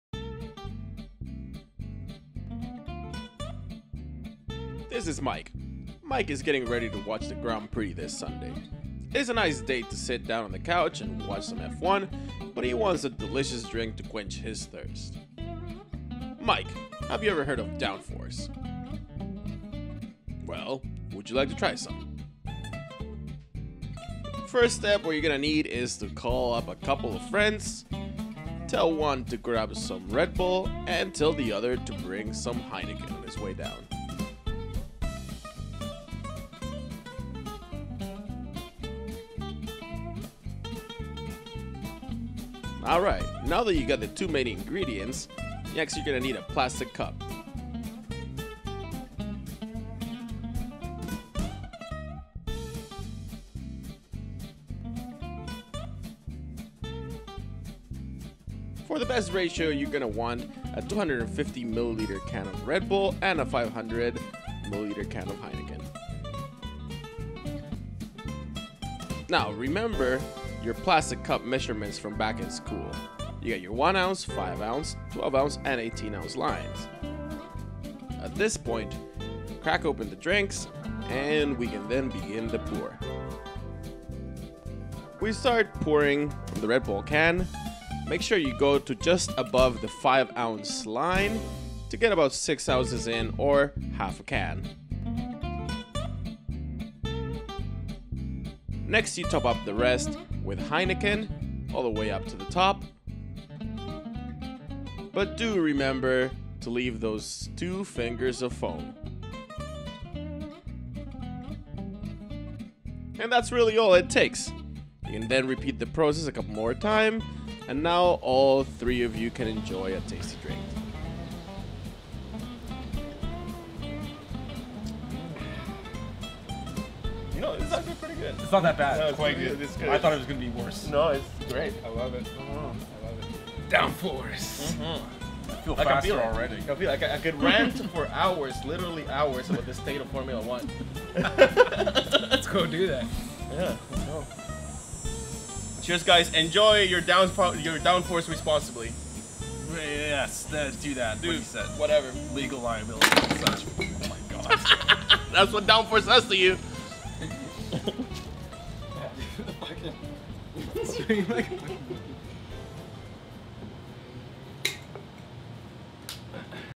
DownForce F1 Drink Instructions
Want to add a bit more DownForce to your day?
Learn how to make a delicious F1-themed beer cocktail.
Here's the secret recipe:
1 part Red Bull, 2 parts Heineken

There you go, that's it. Enjoy responsibly. Don't screw around.